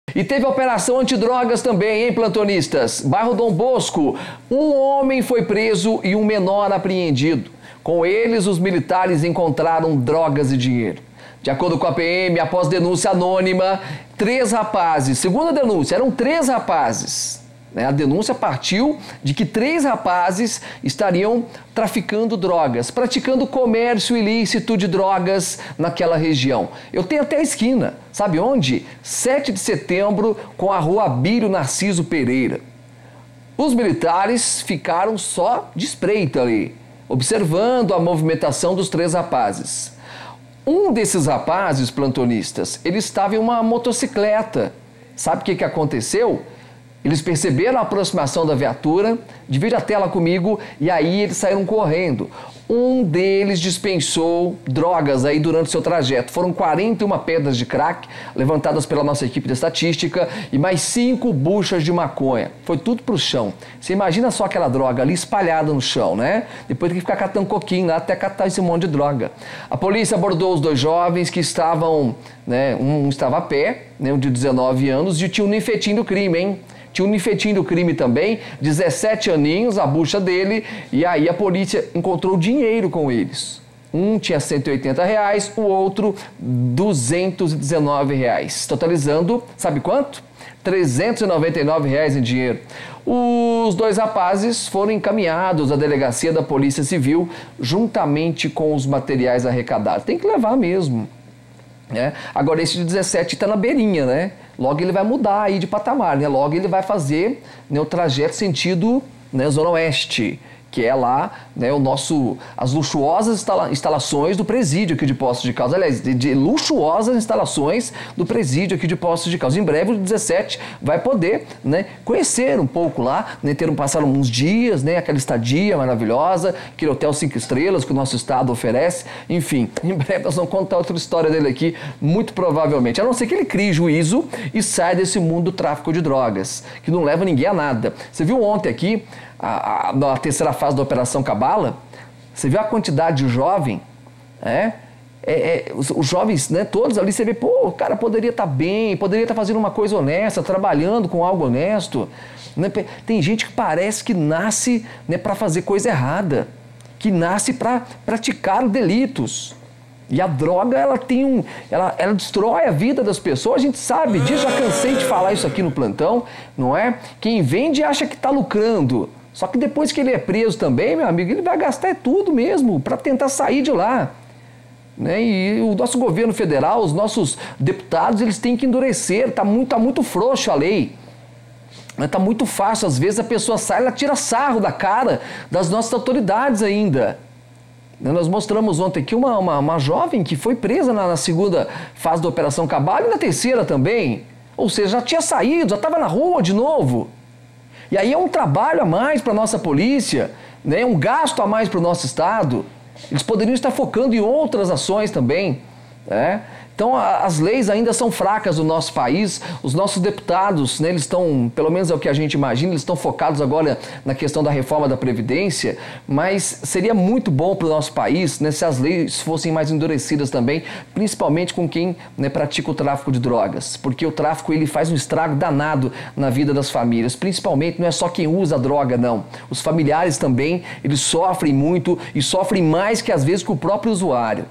OPERAÇÃO NO DOM BOSCO
PM REALIZA OPERAÇÃO ANTIDROGAS NO BAIRRO DOM BOSCO

A operação aconteceu após denúncias anônimas de tráfico de drogas na região. Um homem foi preso e um menor apreendido, com eles foram encontrados drogas e dinheiro.